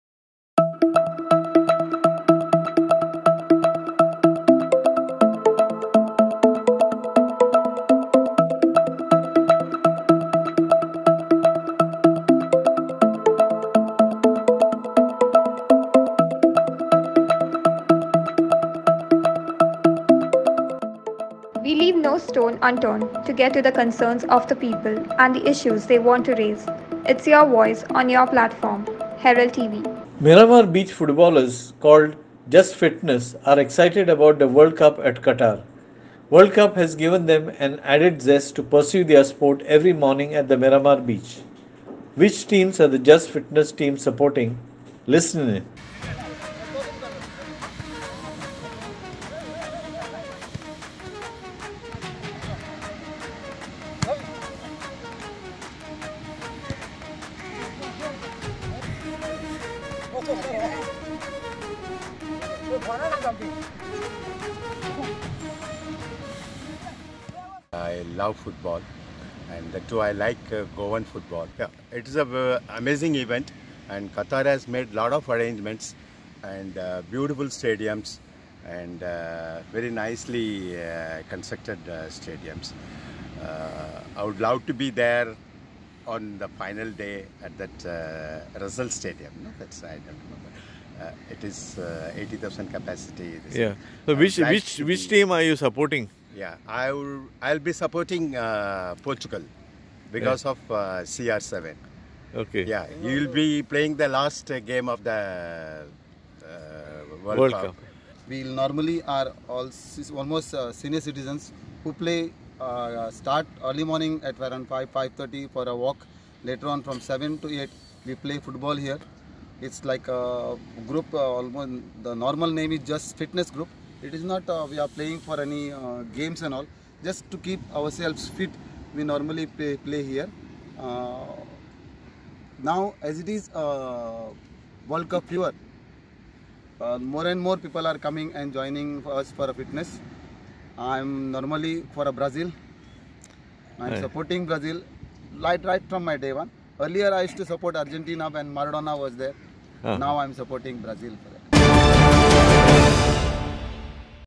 Miramar Beach Footballers “Just Fitness” are excited about the World Cup at Qatar
Miramar Beach Footballers who go by the name of “Just Fitness” and who play football to keep fit are excited about the World Cup at Qatar. The World Cup has given them an added zest to pursue their sport every morning at the Miramar Beach. Which teams are the “Just Fitness” team supporting. Listen in…….